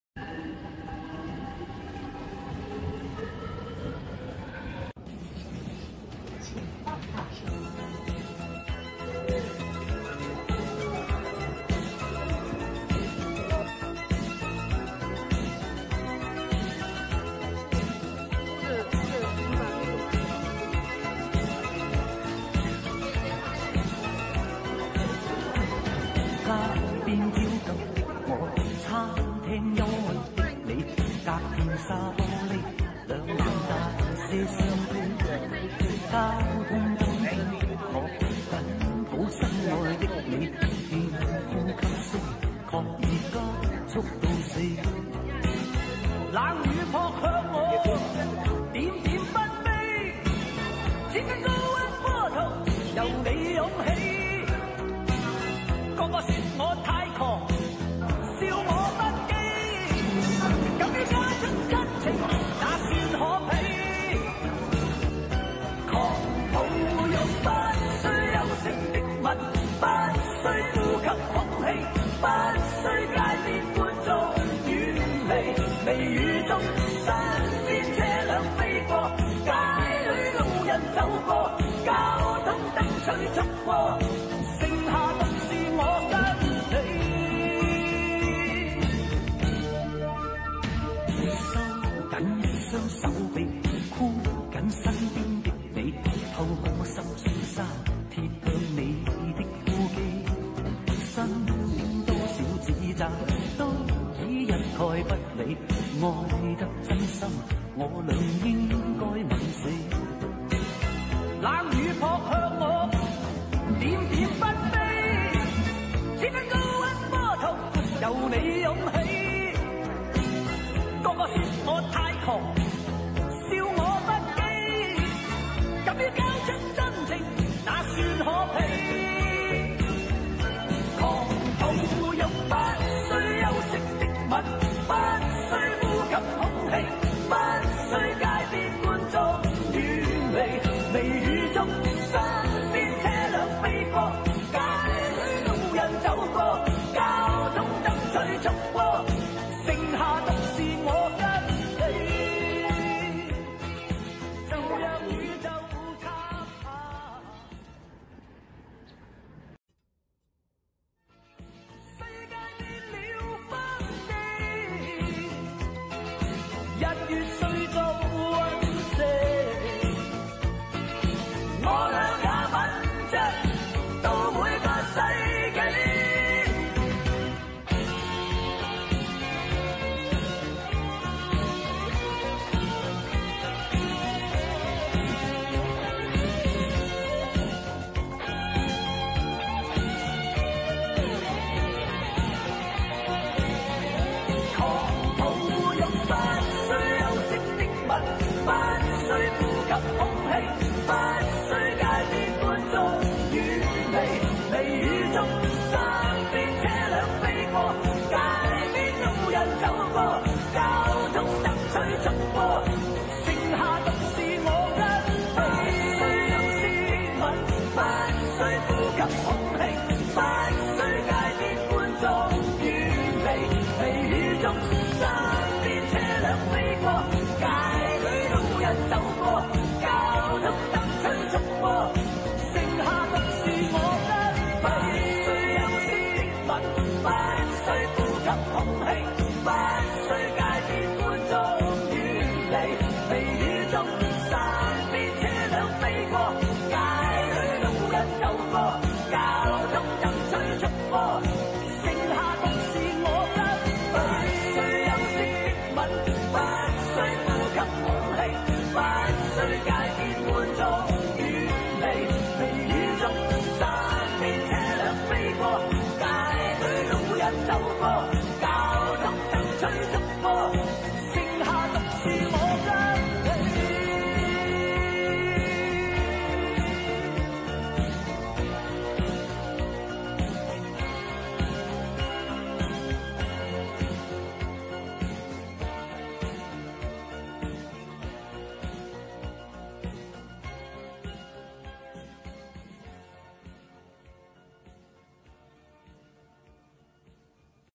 Mini Vlog：周末来咖啡节“大快朵颐”☕️｜治愈时刻 和好朋友一起看《铃芽之旅》🎬｜学习日记 & 饮食记录率
敢爱敢做,今天我是生活的导演,咖啡,vlog,生活记录,治愈,日常,学习,电影,周末,Study With Me,生活万花筒6.0·沸腾吧！青年